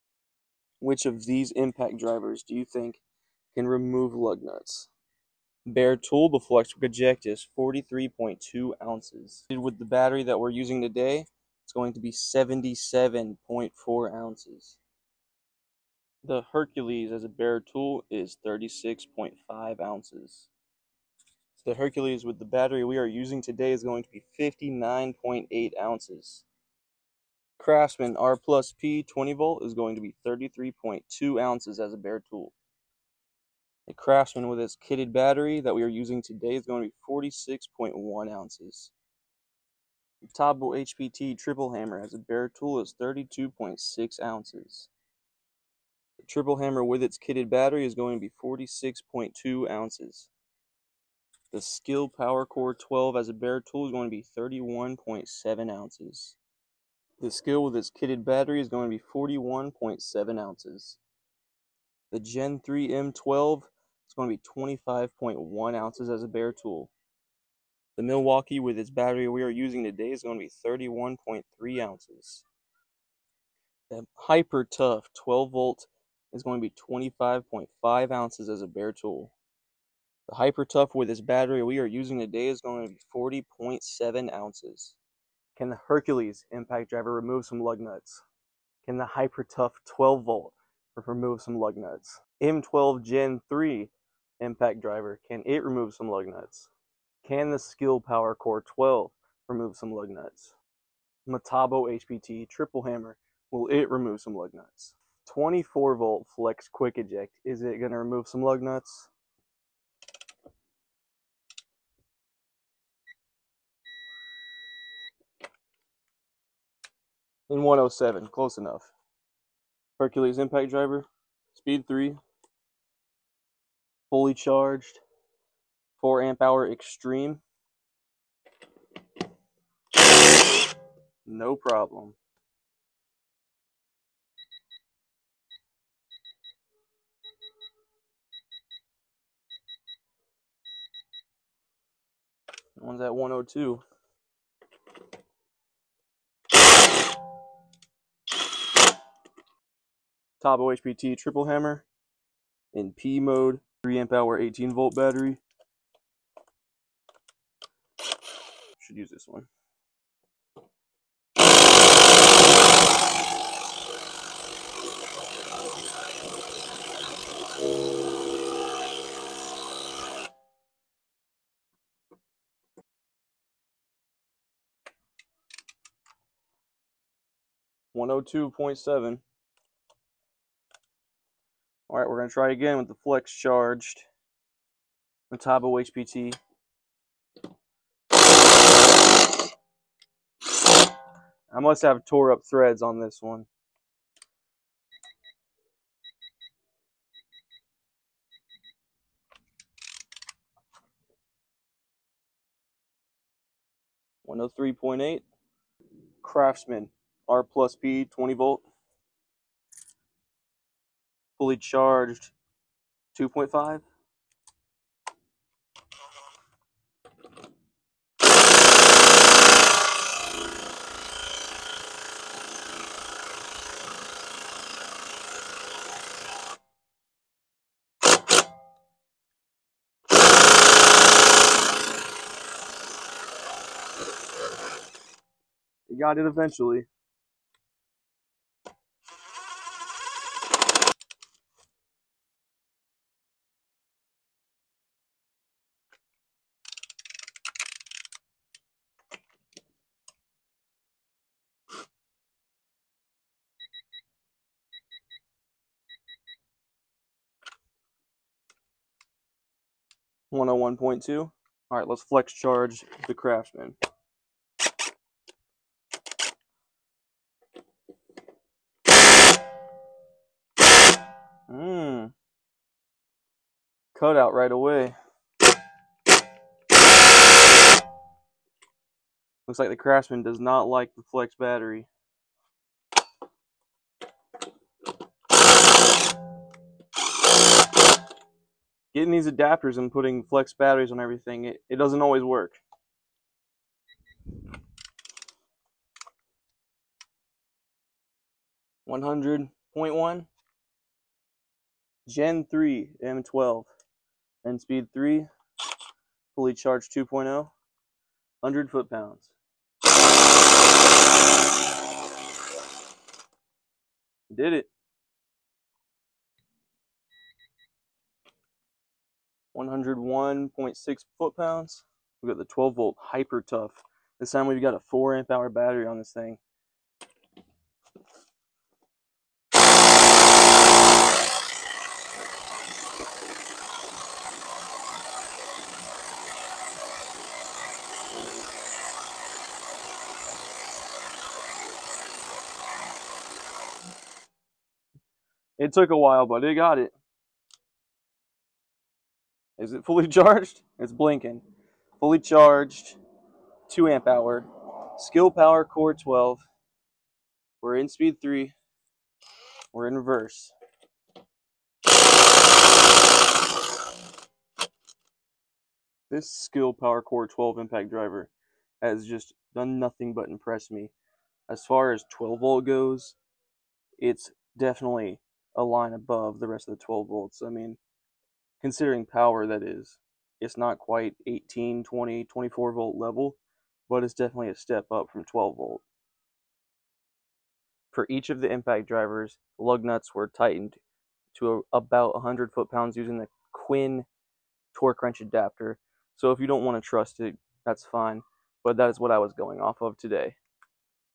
Which of these impact drivers remove LUG NUTS!?
We will test what impact drivers between the Flex quick eject, the Hercules, m12 gen 3, craftsman R+P, SKIL PWRCORE12, Hypertough12v, and Metabo hpt triple hammer can remove lug nuts. Each of them will be torqued to around 100 ft pounds. Have a great day.